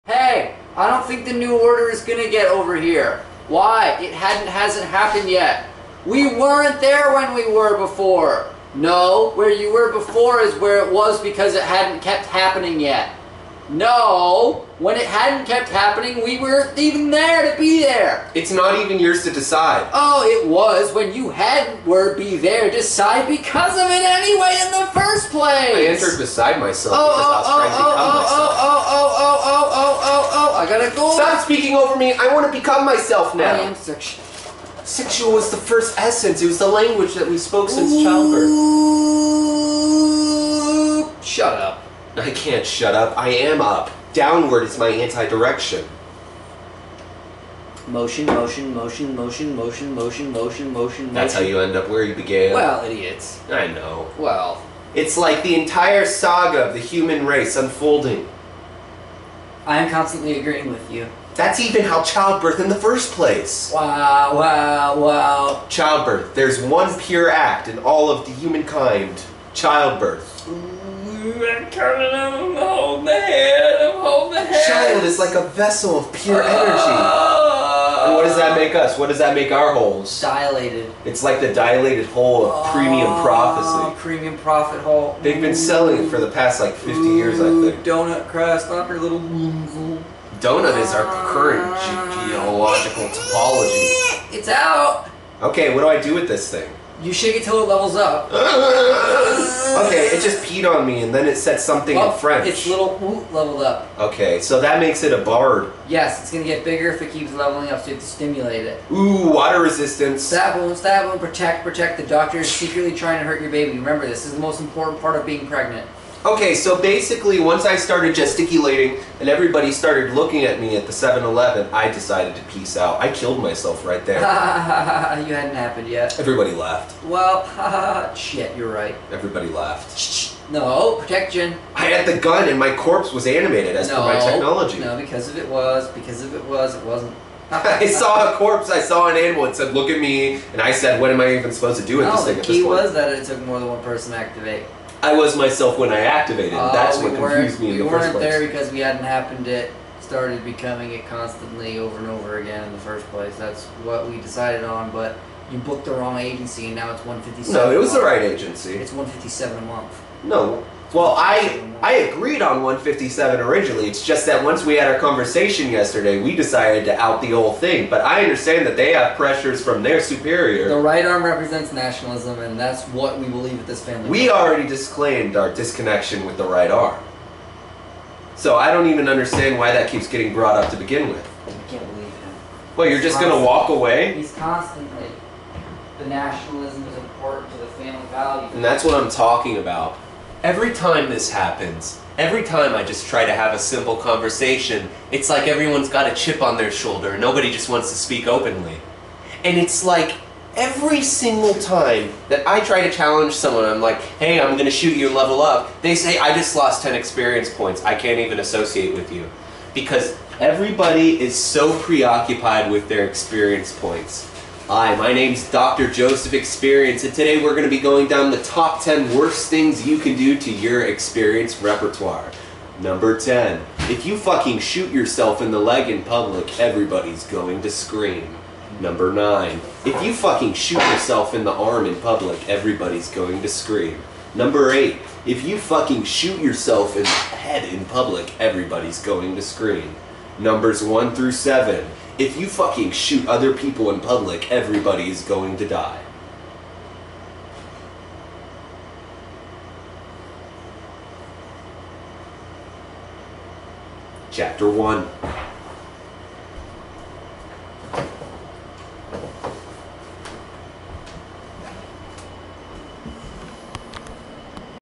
The Conversation | PART XXVI
A rich man and a pauper engage in a civil dialogue regarding philosophy.